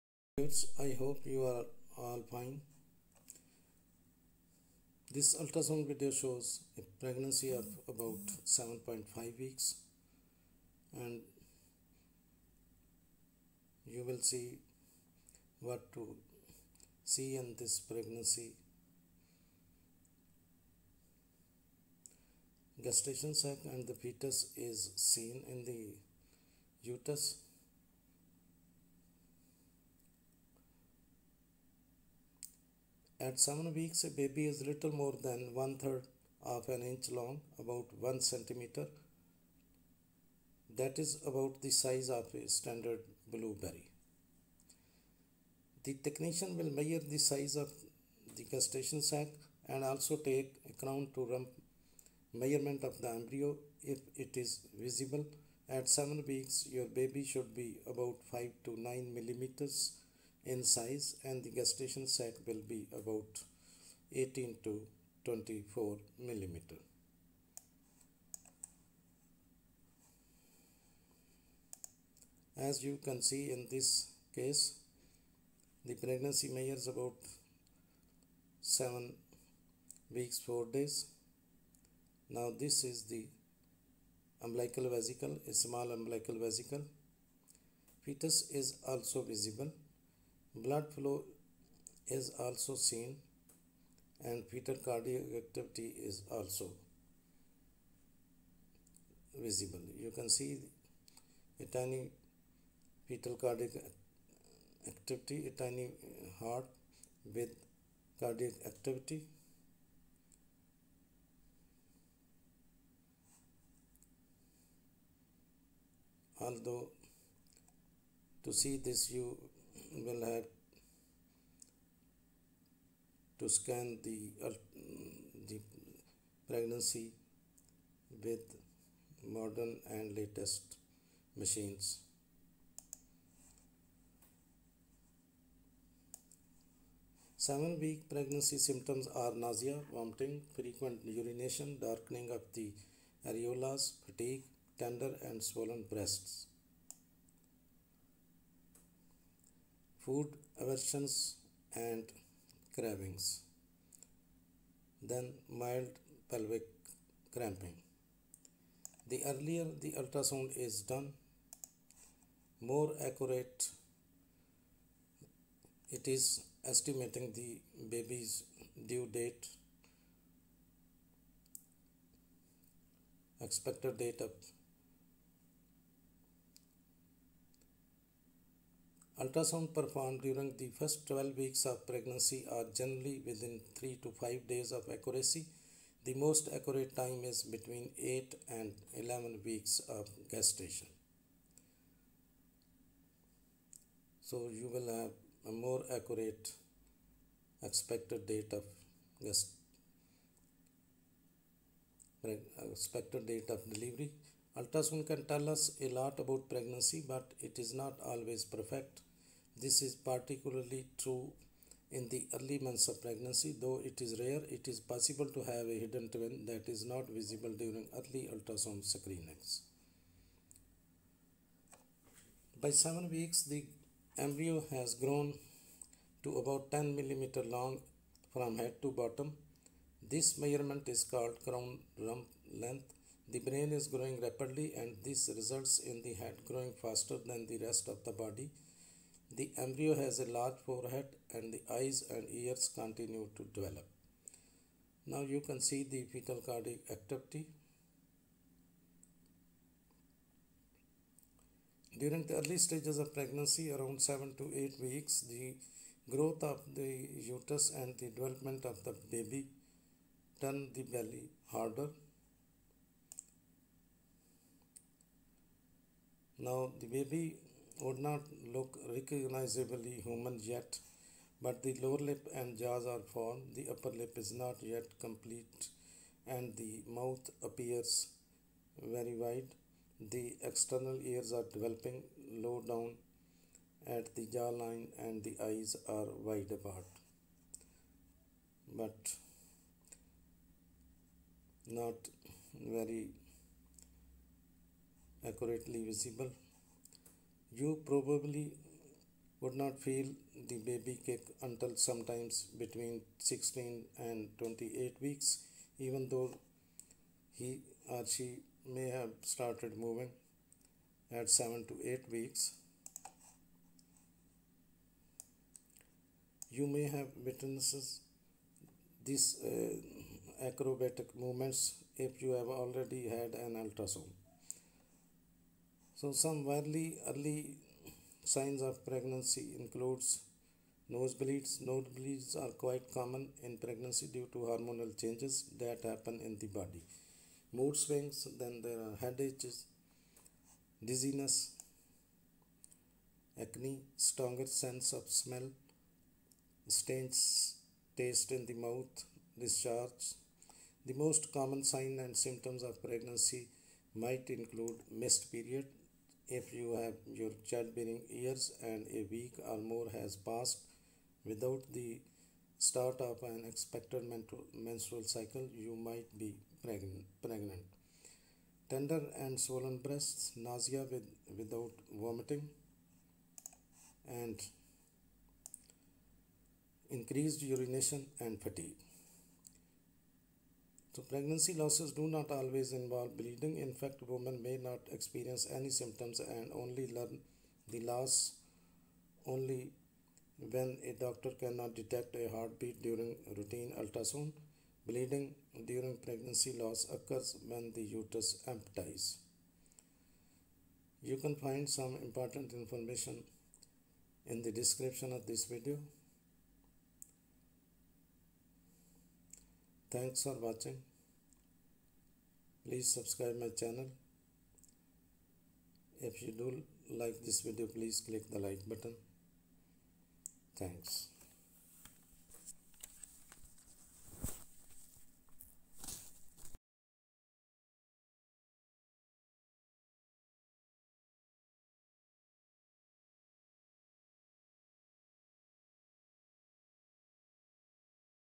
Ultrasound scanning with a Pregnancy of 7.5 weeks.
This video shows Ultrasound scanning with a Pregnancy of 7.5 weeks.
At 7 weeks, a baby is a little more than 1/3 of an inch long (about 1 centimeter). 1 That's about the size of a standard blueberry.
The technician will measure the size of your gestational sac and also take a crown-to-rump measurement of the embryo if it's visible. At 7 weeks, your baby should be about 5 to 9 millimeters (mm) in size and the gestational sac will be about 18 to 24 mm.
7 weeks pregnant symptoms
• nausea.
• vomiting.
• frequent urination.
• darkening of the areolas.
• fatigue.
• tender and swollen breasts.
• food aversions and cravings.
• mild pelvic cramping.
The earlier the ultrasound is done, the more accurate it is at estimating the baby's due date. Ultrasounds performed during the first 12 weeks of pregnancy are generally within 3 - 5 days of accuracy. The most accurate time is between 8 and 11 weeks gestation.
Ultrasound can tell us a lot about a pregnancy, but it's not always perfect. This is particularly true in the early months of pregnancy. Though it is rare, it is possible to have a "hidden twin" that is not visible during early ultrasound screenings.
By 7 weeks, the embryo has grown to about 10mm long from head to bottom. This measurement is called the crown-rump length. The brain is growing rapidly and this results in the head growing faster than the rest of the body. The embryo has a large forehead, and the eyes and ears continue to develop.
During the early stages of pregnancy, around 7 or 8 weeks, the growth of the uterus and the development of the baby, turn the belly harder.
Your baby won't look recognizably human yet, but the lower lip and jaw are formed; the upper lip is not yet complete, and the mouth appears very wide. The external ears are developing low down at the jawline and the eyes are wide apart.
You probably won't feel your baby kick until sometime between 16 and 22 weeks, even though he started moving at 7 or 8 weeks. (You may have witnessed his acrobatics if you've already had an ultrasound.)
Some weird early signs of pregnancy include:
• Nosebleeds. Nosebleeds are quite common in pregnancy due to the hormonal changes that happen in the body. ...
• Mood swings. ...
• Headaches. ...
• Dizziness. ...
• Acne. ...
• Stronger sense of smell. ...
• Strange taste in the mouth. ...
• Discharge.
The most common early signs and symptoms of pregnancy might include:
1. Missed period. If you're in your childbearing years and a week or more has passed without the start of an expected menstrual cycle, you might be pregnant. ...
2. Tender, swollen breasts. ...
3. Nausea with or without vomiting. ...
4. Increased urination. ...
5. Fatigue.
Pregnancy losses do not always involve bleeding. In fact, a woman may not experience any symptoms and only learn of the loss only when a doctor cannot detect a heartbeat during a routine ultrasound. Bleeding during pregnancy loss occurs when the uterus empties.
A pregnancy that doesn't show on an ultrasound scan is called a 'pregnancy of unknown location'. The most common reasons for pregnancy not appearing on the ultrasound scan are: it is too soon to see the baby on the scan.